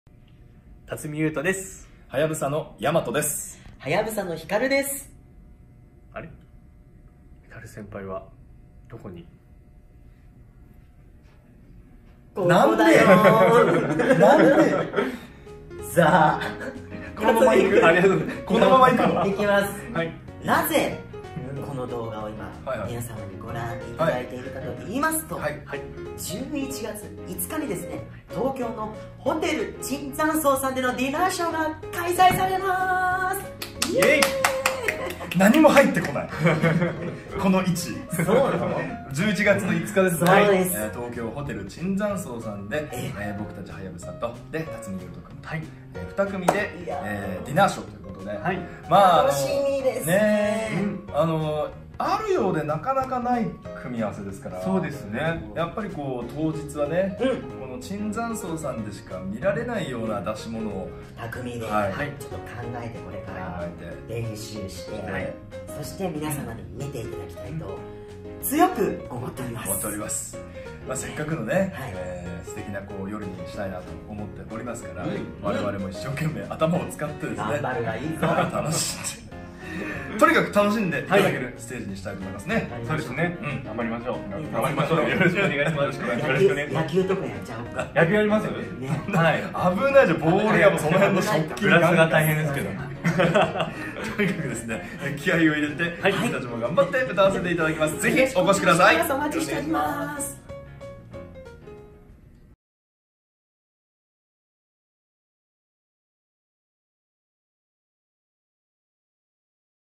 【その１】はやぶさ＆辰巳ゆうとのディナーショーが11月5日開催！ チケット好評発売中！！
はやぶさ＆辰巳ゆうとディナーショー、2019年11月5日(火)開催！

名古屋のディナーショーでも大好評だった2組のディナーショーは必見です！
この機会を逃したら次は無い…かも！？　ぜひご応募ください！

[時間]
18：00　受付開始
18：30　食事開始
20：00　ショー開始
[会場]ホテル椿山荘東京
[料金]￥15,000(税込)
料理・飲物・ショーチャージ・サービス料・消費税込
[お問合せ]
(ホテル椿山荘東京予約センター9：00～20：00)
詳細はコチラ